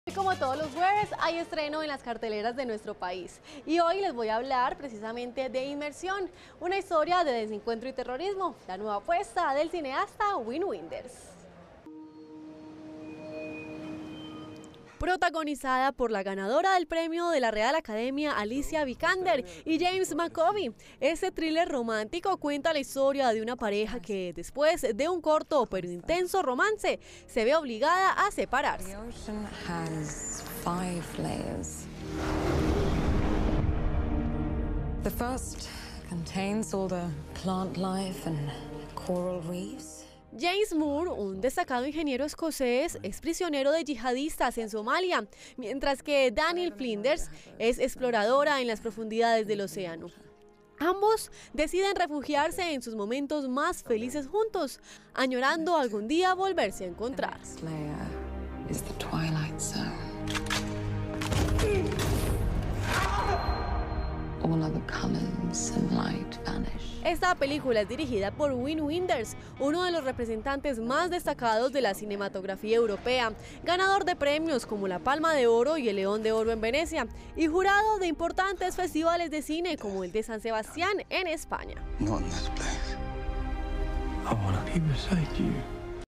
Inmersión, nuestra película recomendada, un corto pero intenso romance [Noticias] - Telemedellín
Esta película es dirigida por Win Wenders, uno de los representantes más destacados de la cinematografía europea.